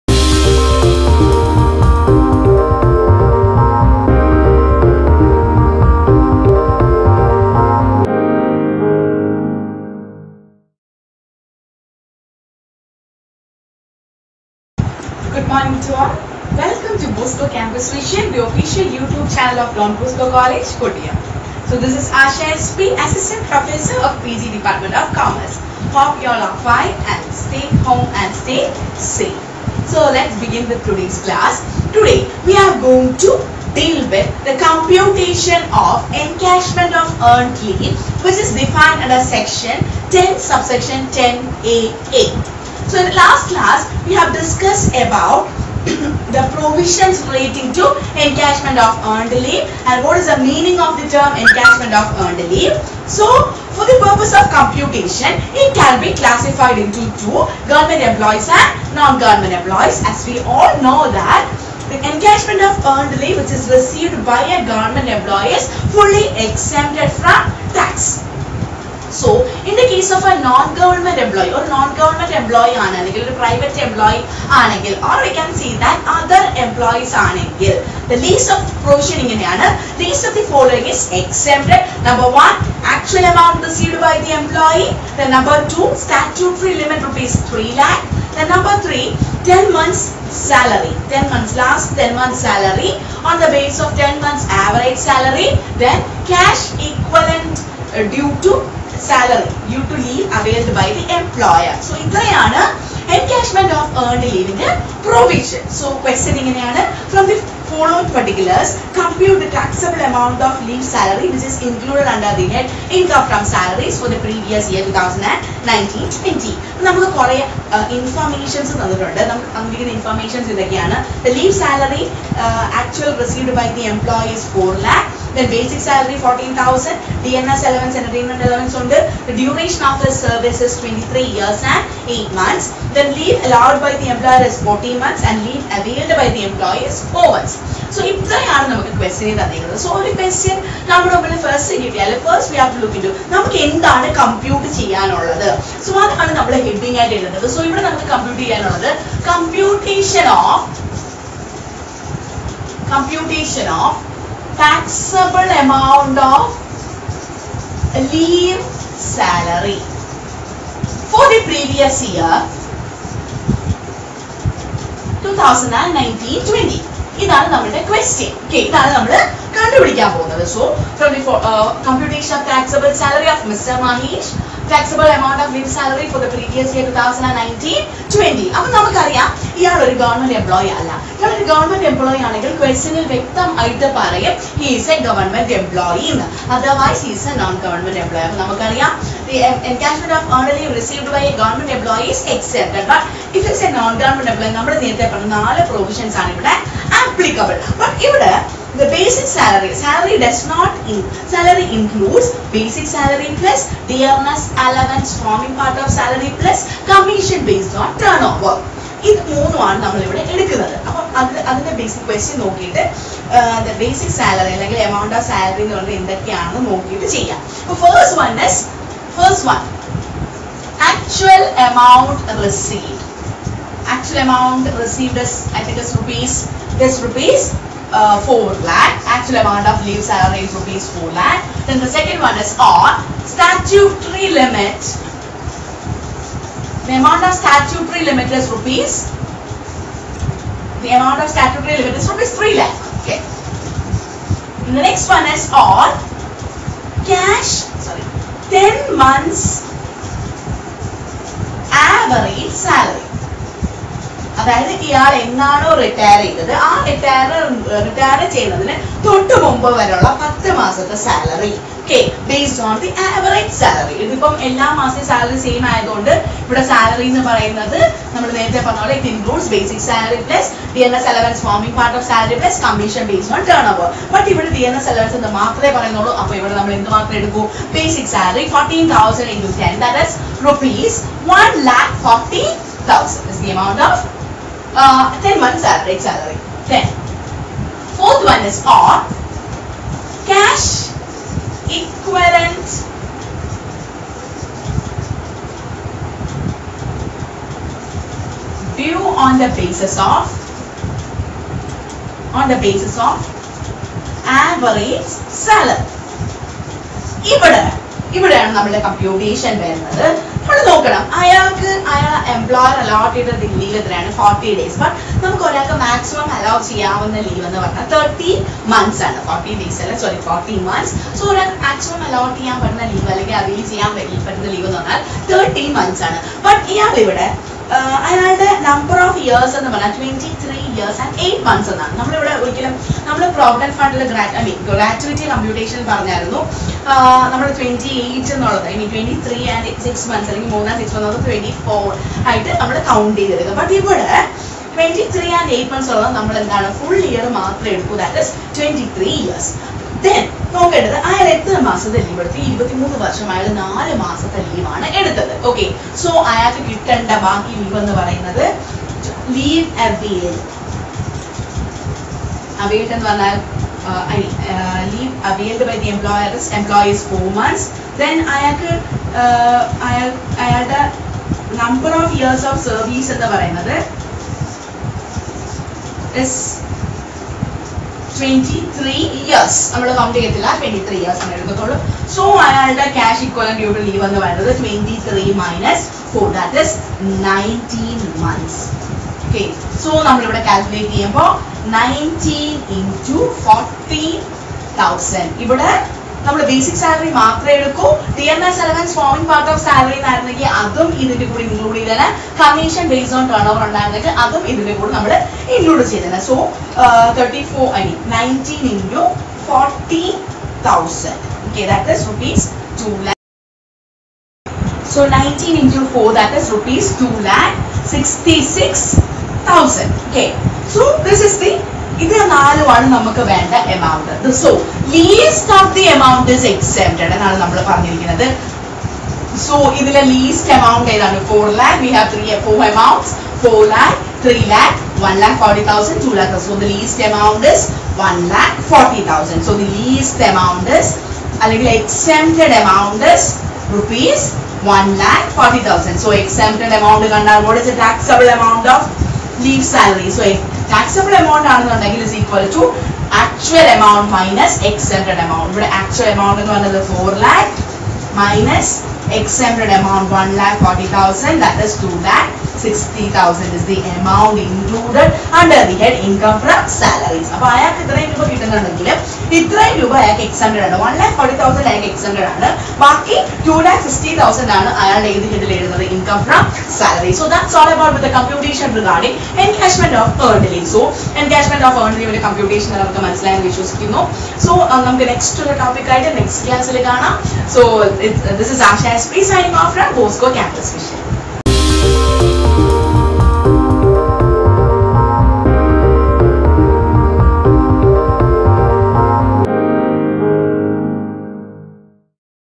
Computation of Encashment of earned leave : Lecture 19
The computation of encashment of leave is illustrated with a worked out example.